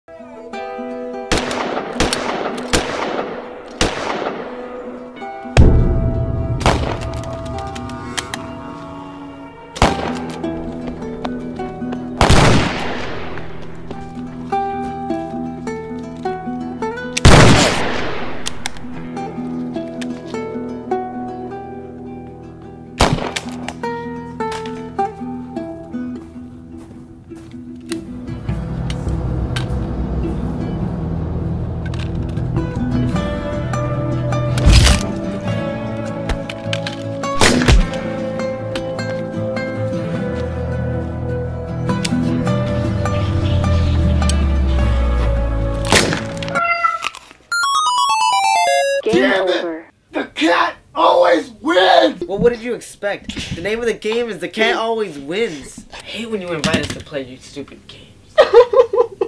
THE SHOOTOUT
Never leave your guard down in this kind of shootout.

This is our entry video for Film Riot's "Monday Challenge" contest!